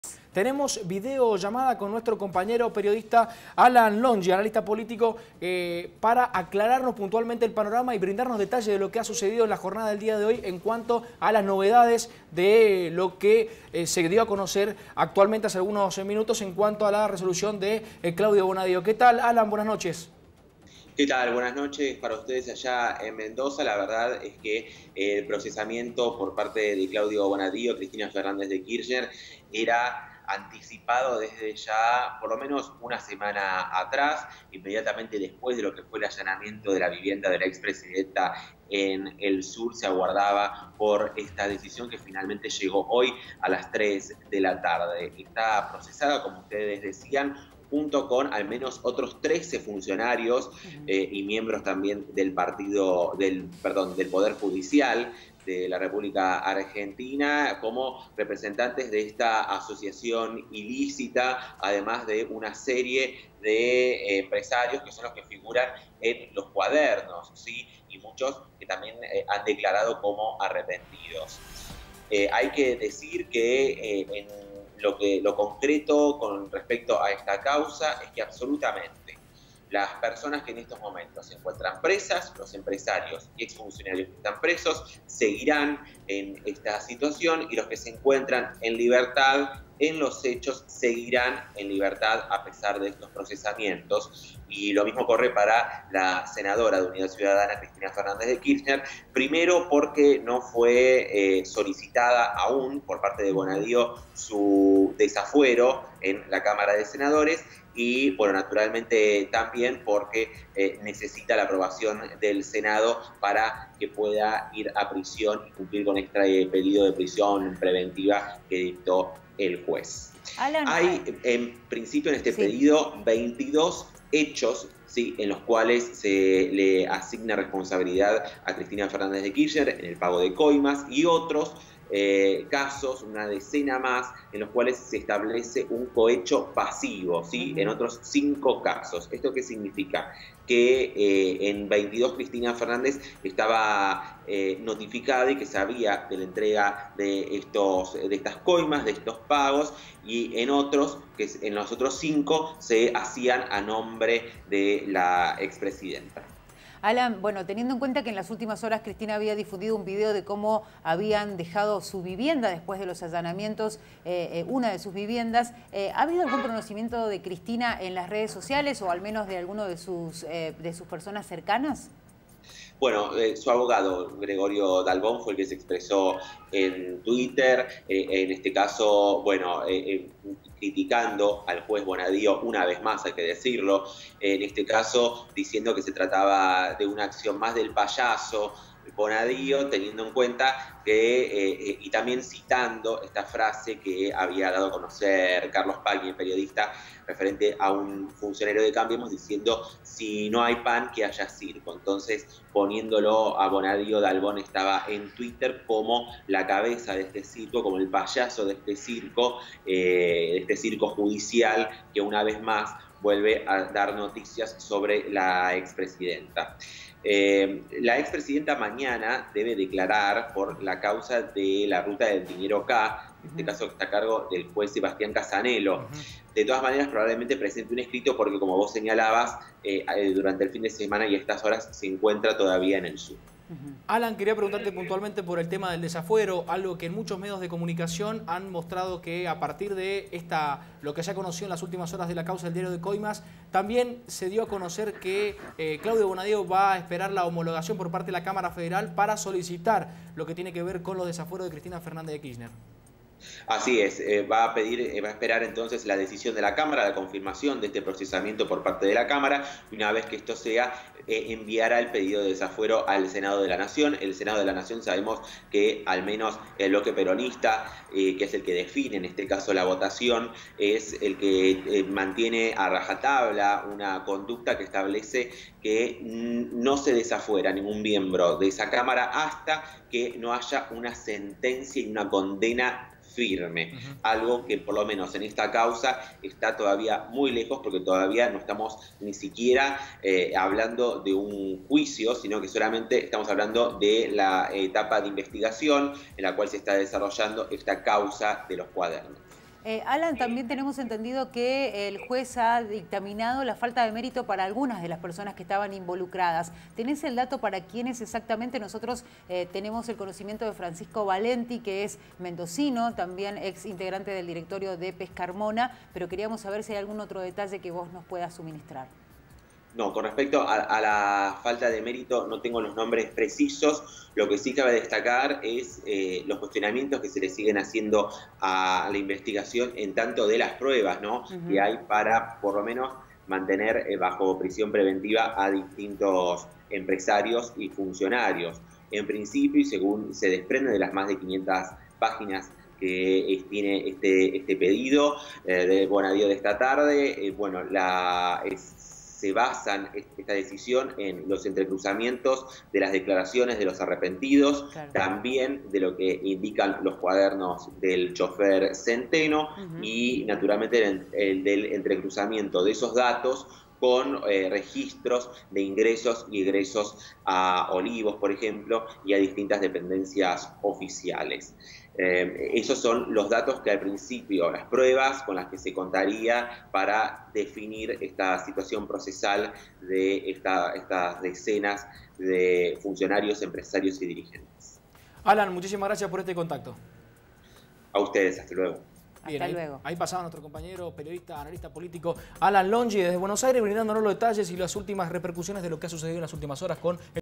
Bonadio procesó con prisión preventiva a CFK
Comunicación telefónica con Alan Longy (Analista Político).